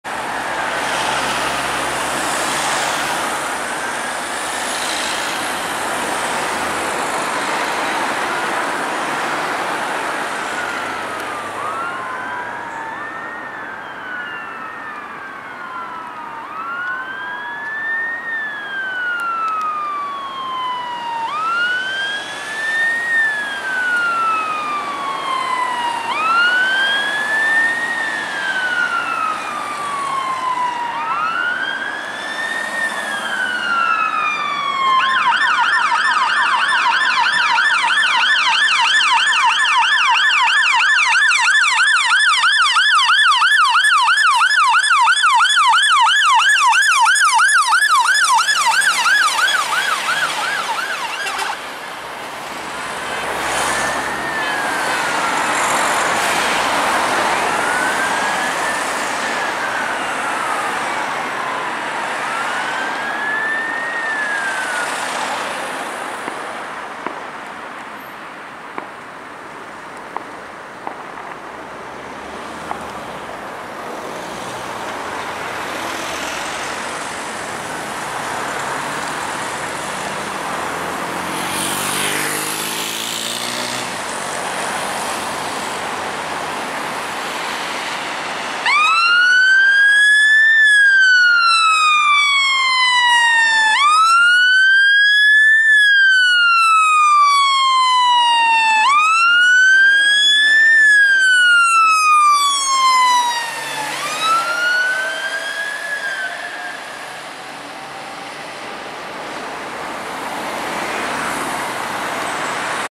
Merseyside Fire & Rescue Service - Croxteth Search & Rescue Team Responding
Merseyside Fire & Rescue Service - Croxteth Search & Rescue Team (SRT) Responding to a Road Traffic Collision (RTC) in Huyton earlier this evening, 11/11/2017, on Blue Lights and Wailers